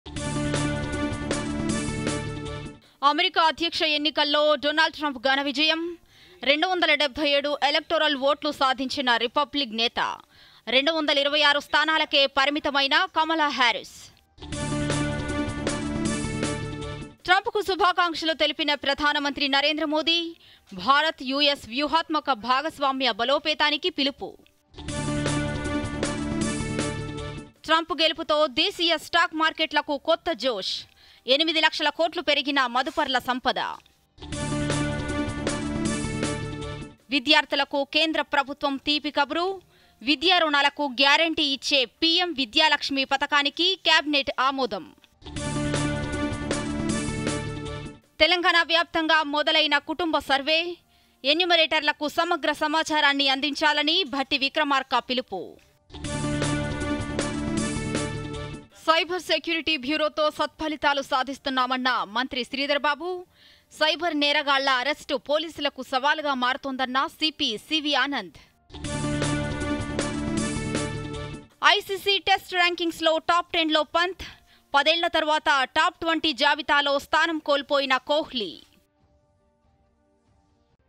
headlines | DD News Telangana | తెలంగాణ న్యూస్
అమెరికా అధ్యక్ష ఎన్నికల్ల్లో ట్రంప్‌ ఘన విజయం
ట్రంప్ కు శుభాకాంక్షలు తెలిపిన ప్రధానమంత్రి నరేంద్ర మోదీ
ట్రంప్ గెలుపుతో స్టాక్ మార్కెట్లకు కొత్త జోష్
విద్యార్థులకు కేంద్ర ప్రభుత్వం తీపికబురు
తెలంగాణ వ్యాప్తంగా మొదలైన కుటుంబ సర్వే
సైబర్ సెక్యూరిటీ బ్యూరోతో సత్ఫలితాలు సాధిస్తున్నామన్న మంత్రి శ్రీధర్ బాబు
ఐసీసీ టెస్ట్ ర్యాంకింగ్స్ లో టాప్ టెన్ లో పంత్